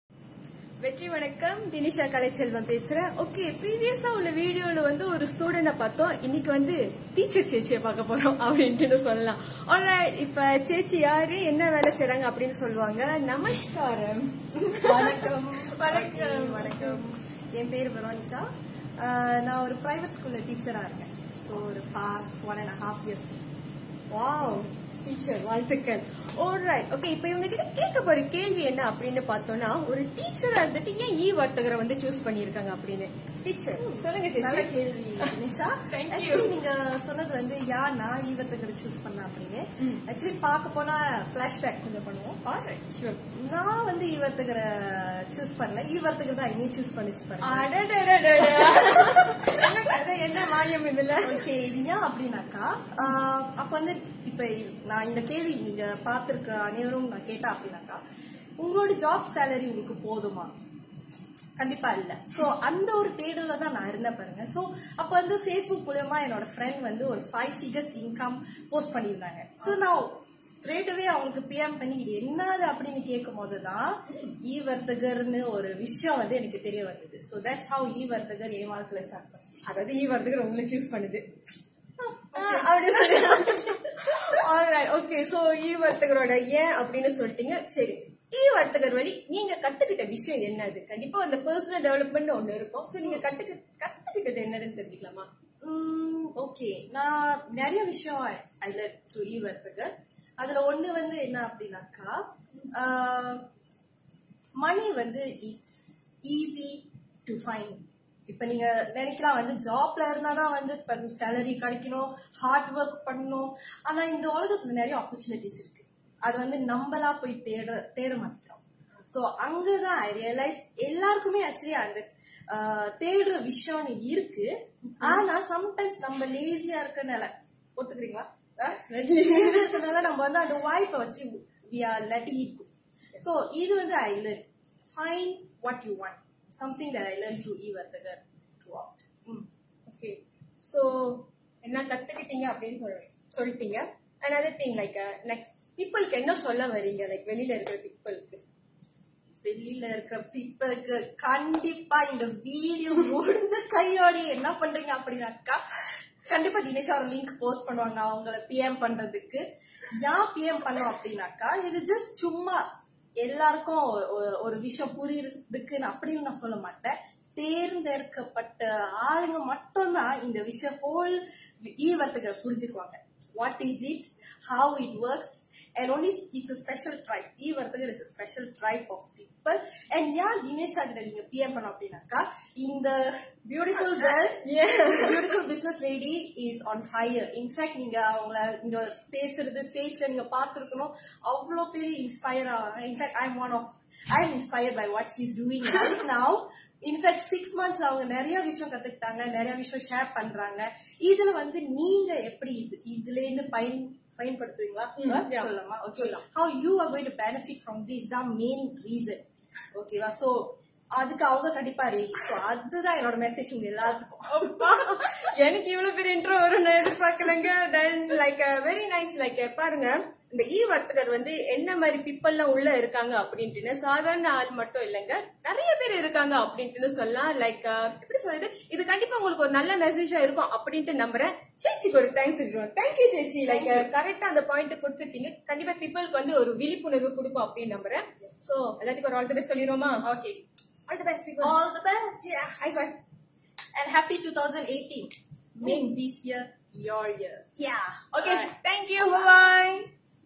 eVarthagar girl Power - chit chat with eVarthagar Dineesha & eVarthagar WBC Veronica
A Small Shoot With TEACHER Entrepreneur !
Why she choose eVarthagar ? Ops why eVarthagar choose her ? Haha..
What she learn from eVarthagar ?
Check out this video guys 😎 Energetic Woman Entrepreneur